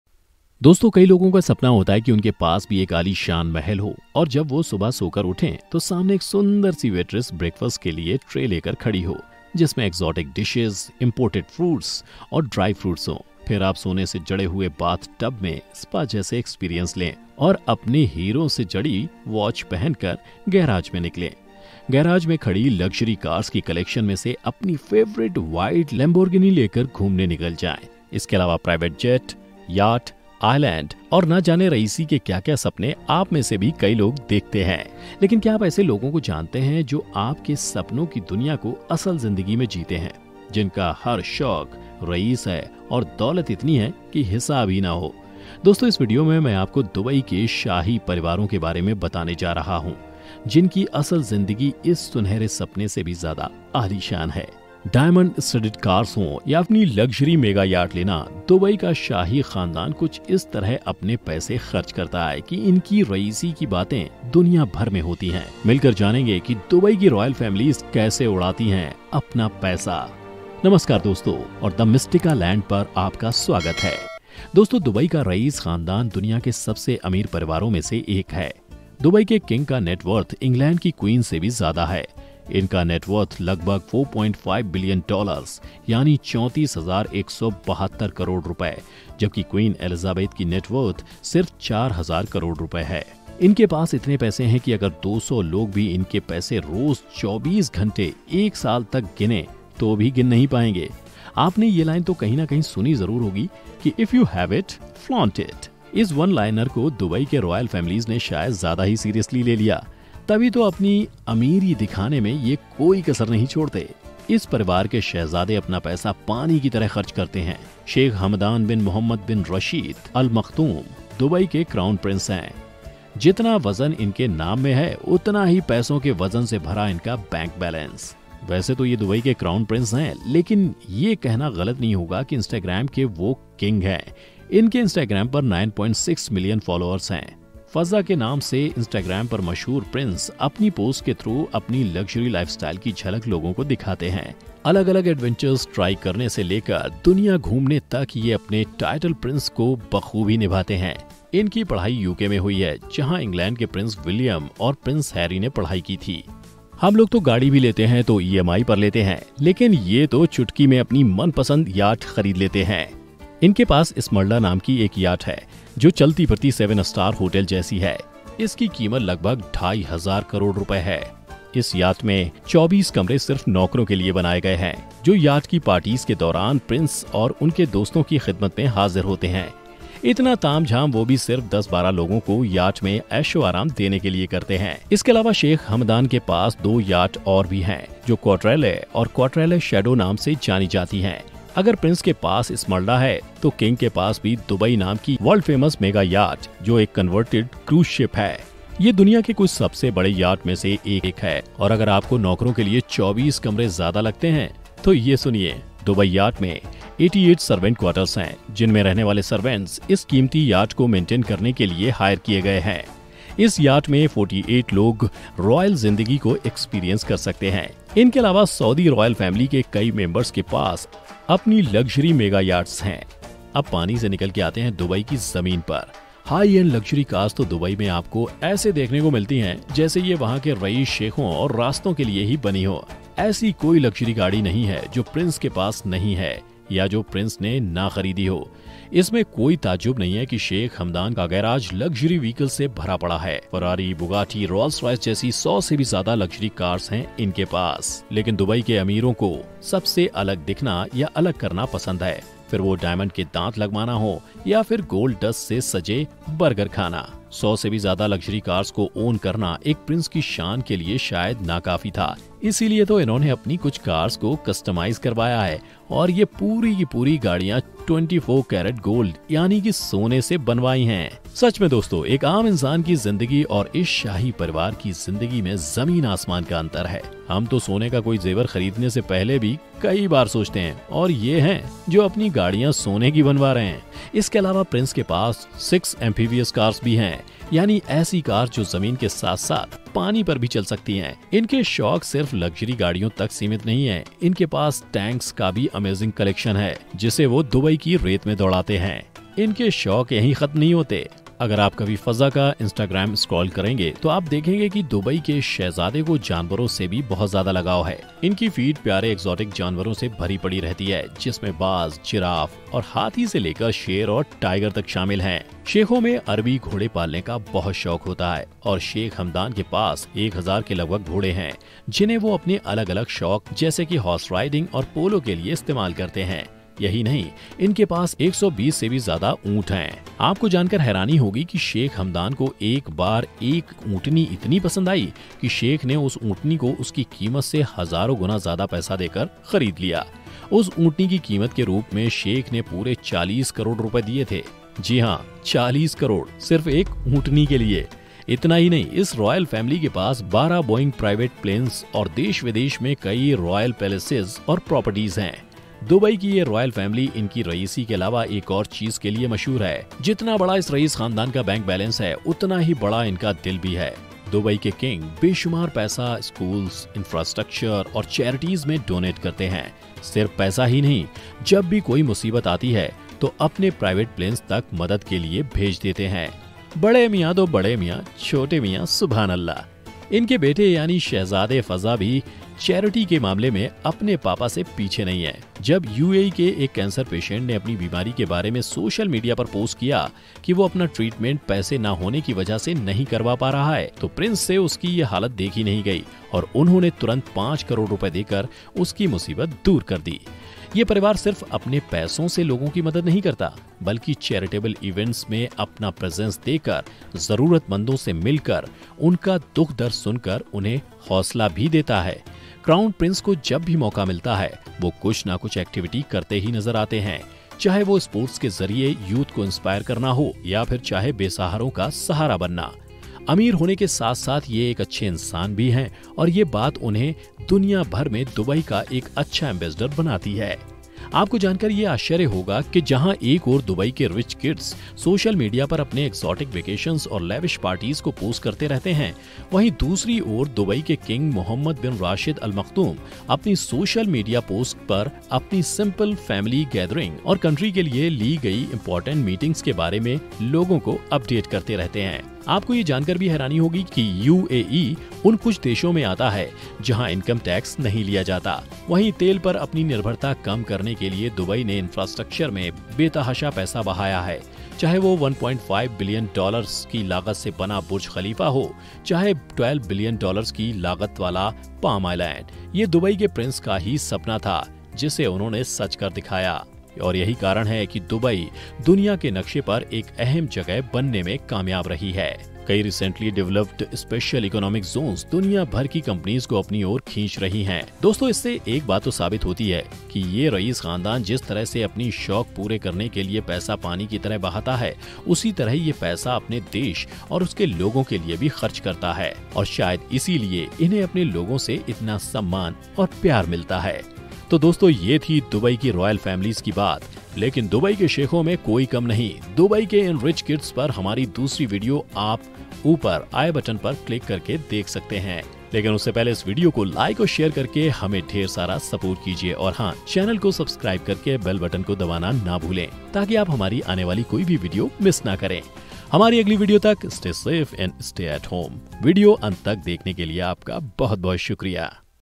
दुबई का शाही परिवार अपनी दौलत कहाँ खर्च करता है? | How Royals of Dubai spend their Billions?
Video Title : दुबई का शाही परिवार अपनी दौलत कहाँ खर्च करता है? | How Royals of Dubai spend their Billions?
Language  : Hindi